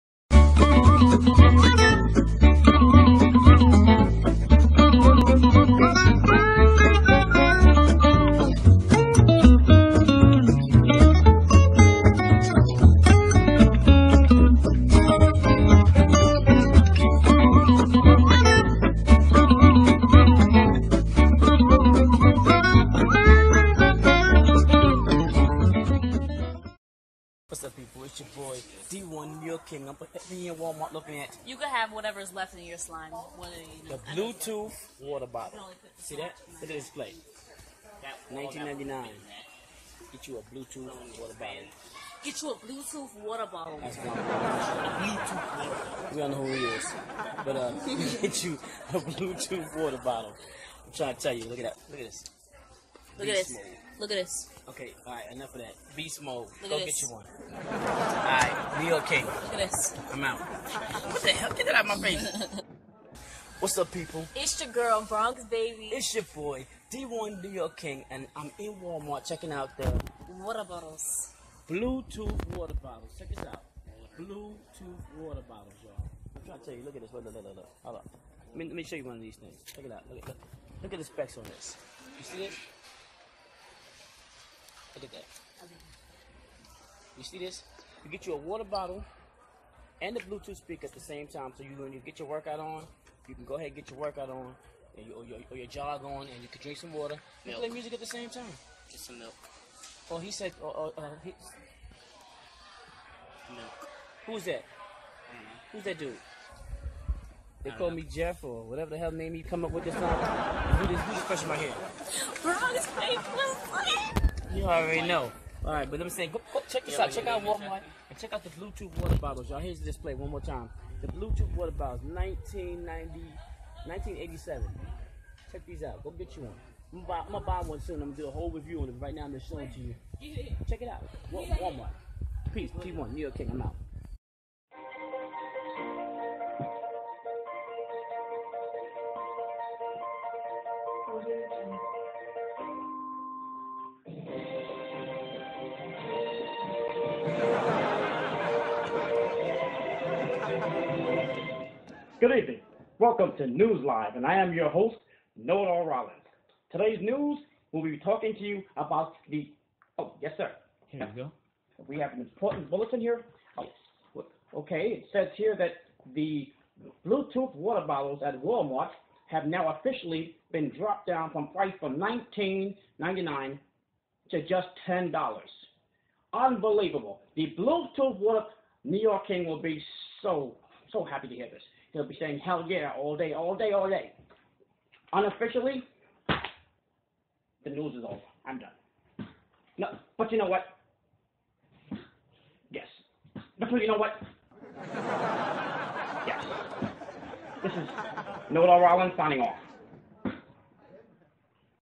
Bluetooth speaker water bottles at Walmart.
I came across these water bottles at Walmart and had to make this video. Go check your local Walmart for one.